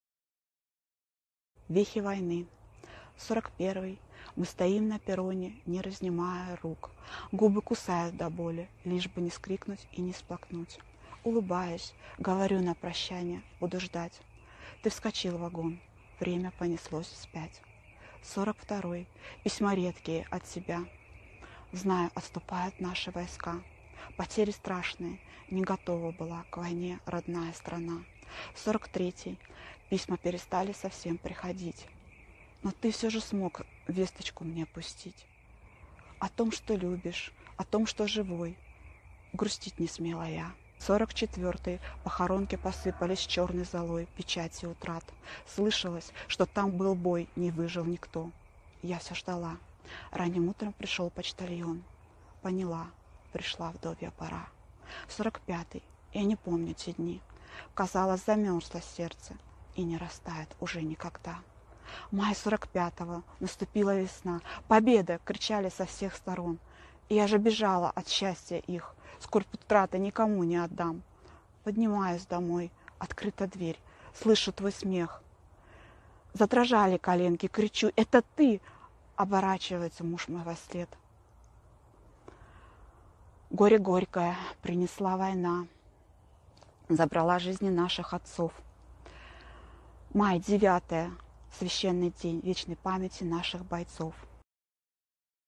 Вехи войны | Автор стихотворения: Юлия Лилея
Мы стоим на перроне,
Не разнимая рук.
Сжимаю губы до боли,
Лишь бы не крикнуть
И не всплакнуть.
Улыбаюсь, говорю
На прощание: буду ждать.
Ты вскочил в вагон,
Время понеслось вспять.
42
Письма редкие от тебя.
Знаю, отступают наши войска.
Потери страшные.
Не готова была к
Войне родная страна.
43
Перестали письма
Совсем приходить.
Только смог же ты
Весточку передавать.
Что живой, что любишь.
Грустить, не смела я.
44
Похоронки посыпались
Чёрной золой, печатью утрат.
Слышалось там был бой -
Не выжил никто.
Я всё ждала.
Ранним утром пришел
Почтальон, поняла -
Наступила вдовья пора.
45
Я не помню те дни.
Казалось замёрзло
Сердце и не расстает
Уже никогда.
Май 45
Пришла весна. Победа!
Кричали со всех сторон.
Я ж бежала от счастья их.
Скорбь утраты никому не отдам.
Поднимаюсь домой,
Открыта дверь.
Слышу твой смех.
Задрожали коленки,
Кричу: Это ты?
Оборачивается муж
Мой во след.
Горе горькое принесла
Война, забрала жизни
Сотни отцов.
Май, девятое - священный день
Вечной памяти наших бойцов!